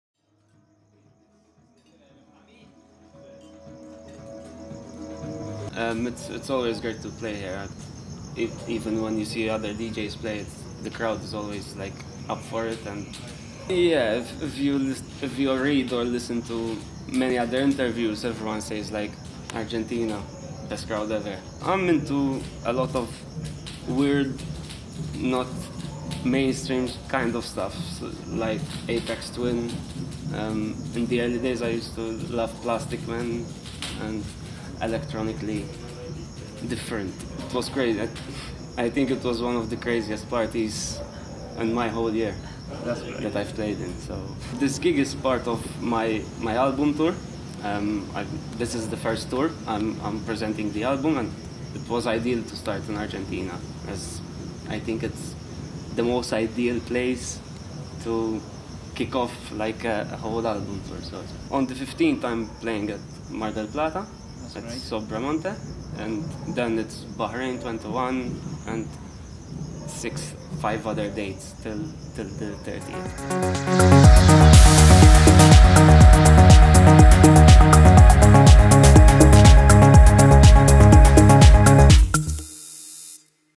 Deepfunk Interview - Sindor Producciones - Córdoba, Argentina 2014
Entrevista a Deepfunk, horas antes de su performance en Maria Cena de Sindor Records, en Córdoba, Argentina